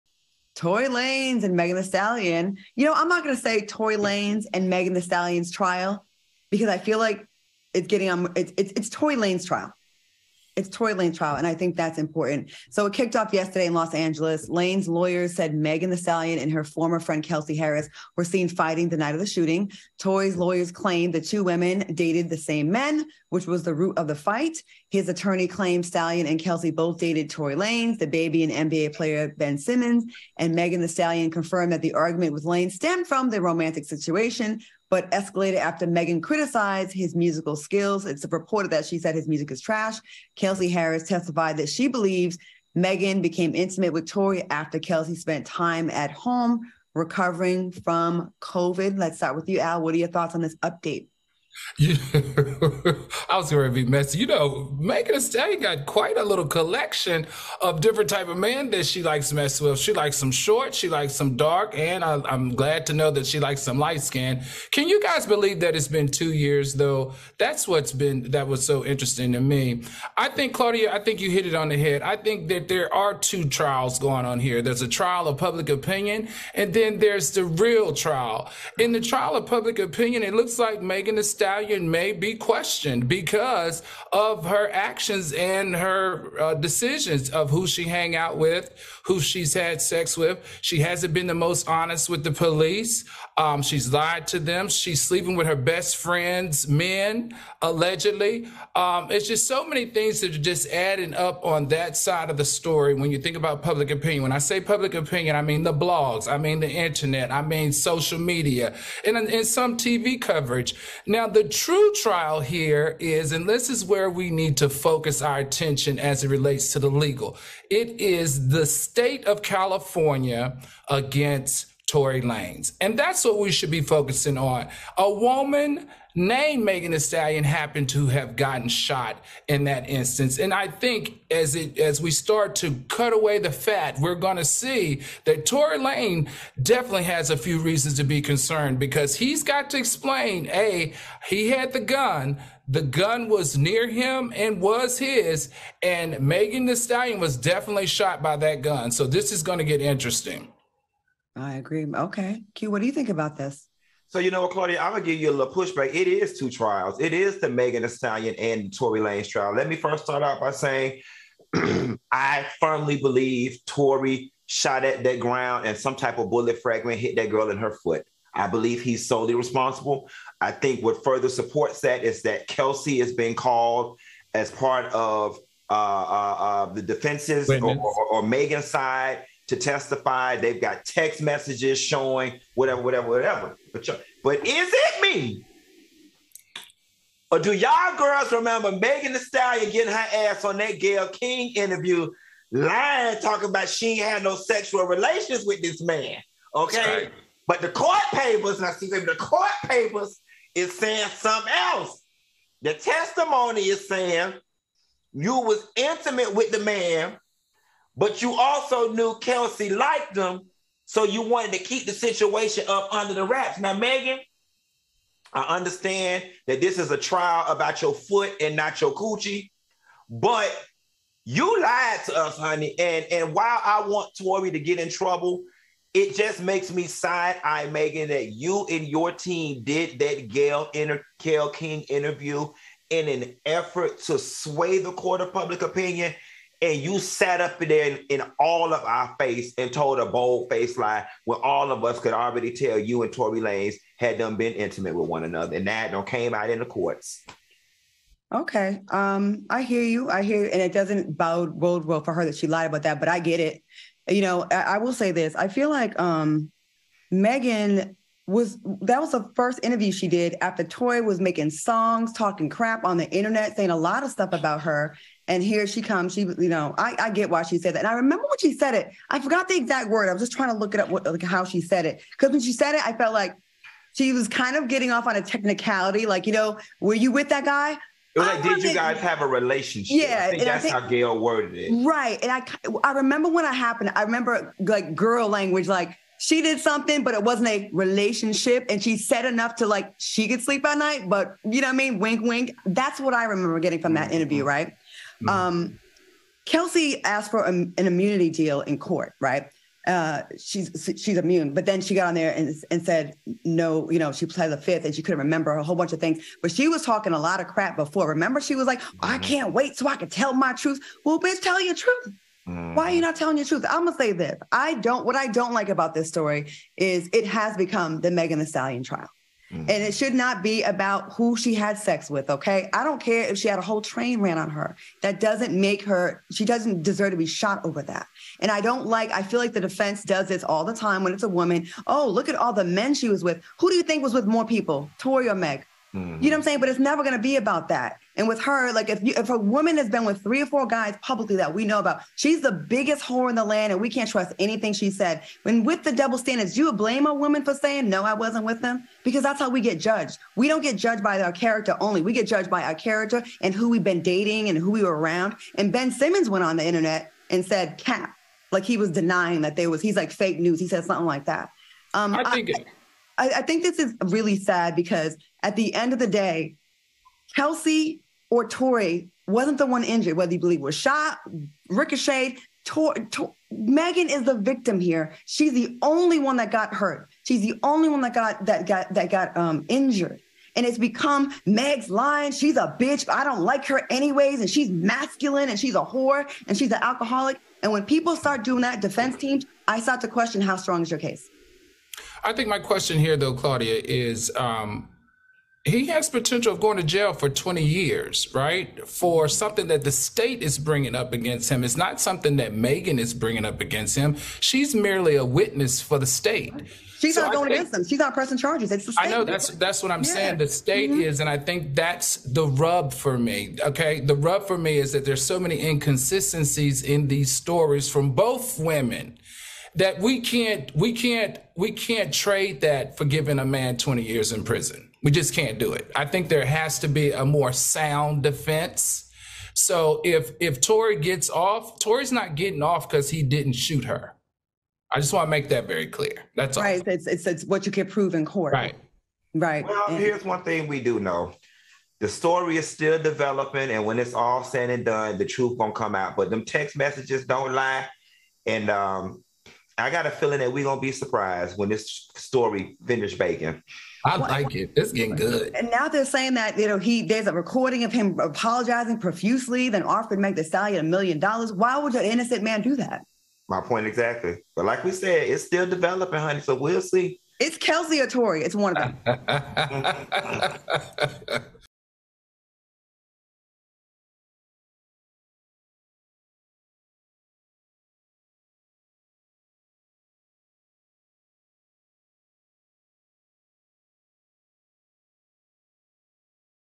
Tory Lanez, Megan Thee Stallion Trial Heats Up! | Tea-G-I-F
Claudia, Al and Funky are talking about the Tory Lanez trial and who is telling the truth!